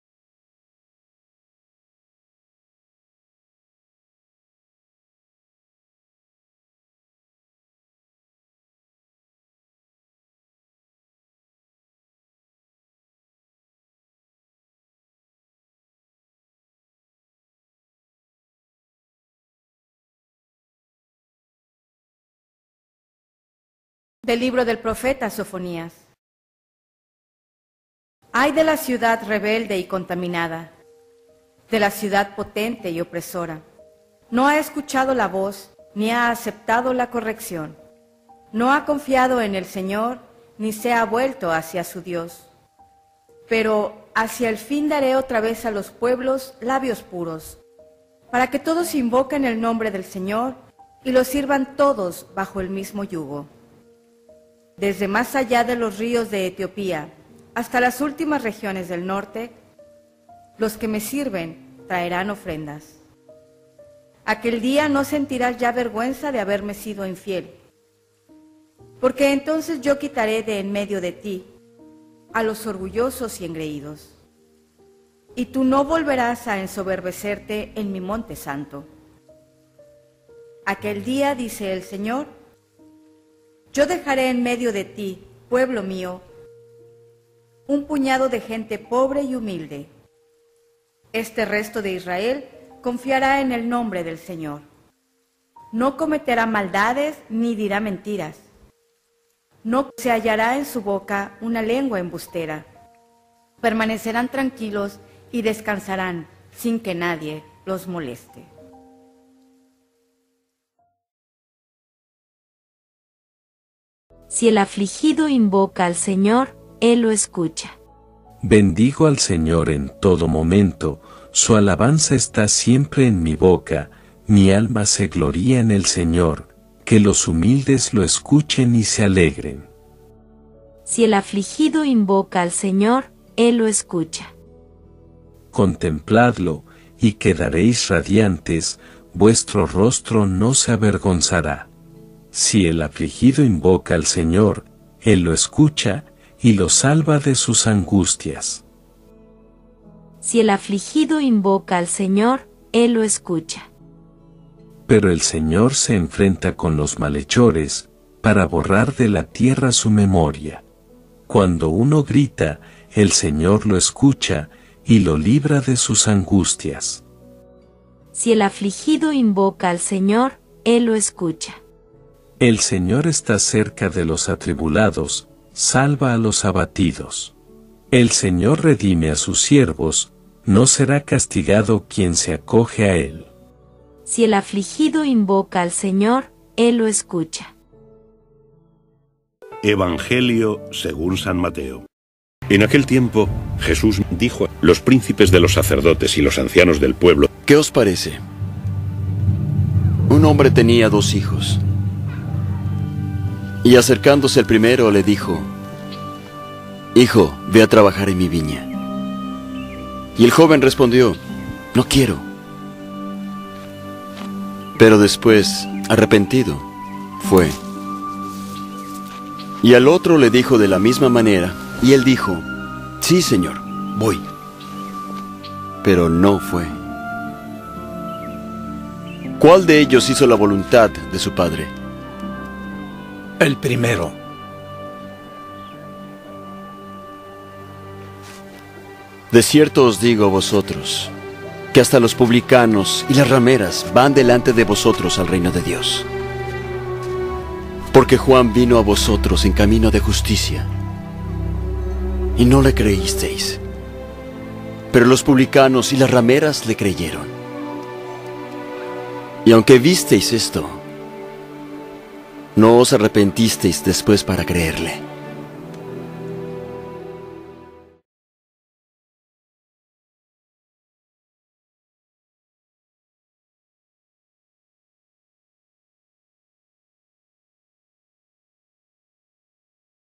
EVANGELIO LECTURAS MARTES 13 DE DICIEMBRE DE 2022 | MATEO 21,28-32 | MARTES III SEMANA DE ADVIENTO
EVANGELIO Y LECTURAS EUCARISTÍA MARTES 13 DE DICIEMBRE DE 2022 MARTES TERCERA SEMANA DEL TIEMPO DE ADVIENTO CICLO A Primera Lectura: Del libro del profeta Sofonías 3,1-2.9-13 Salmo 34(33),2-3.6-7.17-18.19 y 23 R/.Si el afligido invoca al Señor, él lo escucha EVANGELIO Según San Mateo 21,28-32